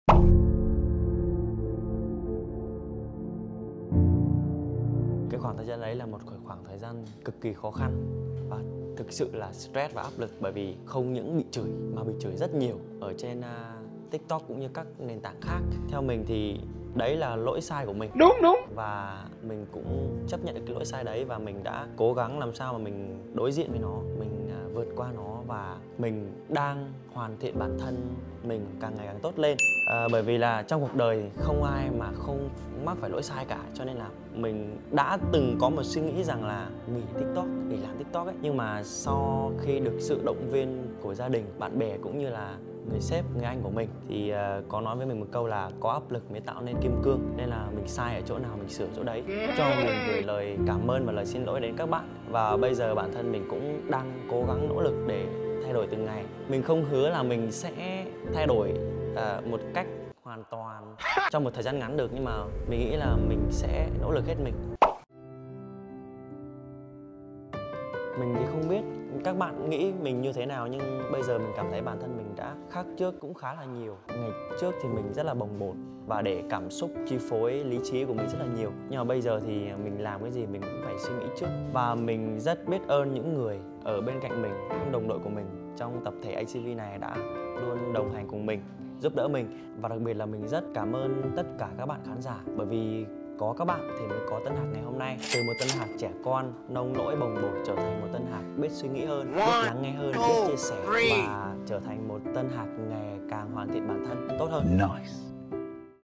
Hỏi nhanh đáp nhanh | Tân Hạc đã thay đổi thế nào sau những sai lầm trong quá khứ | ACV GenZ
Cùng lắng nghe những trải lòng của tiktoker Tân Hạc về những câu chuyện xảy ra trong quá khứ và chứng kiến sự thay đổi của Tân ở thời điểm hiện tại ra sao?
Mọi người cùng đăng kí hệ thống của ACV GenZ nha:
© Bản quyền thuộc về ACV Entertainment
© Copyright by ACV Entertainment☞ Do not Reup"